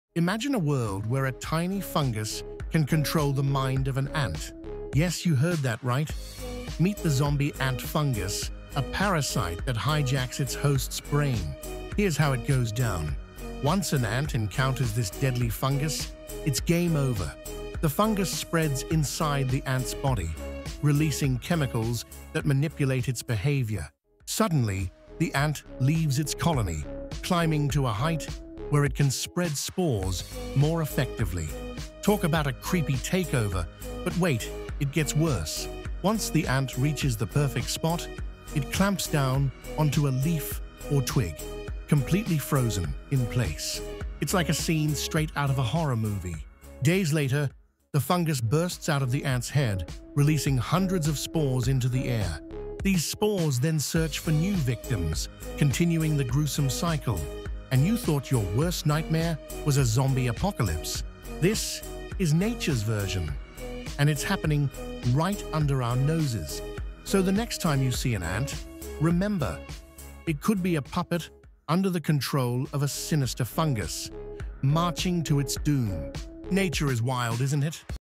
Zombie Ant Fungus!? 🍄🧟‍♂️🐜
imagine an ant losing control of its own body. a parasitic fungus takes over its brain, forcing it to climb, bite down, and wait… until the fungus bursts out, spreading spores to the colony. nature’s real-life zombie apocalypse is already here — and it starts with ants. 🧟‍♂️🐜🍄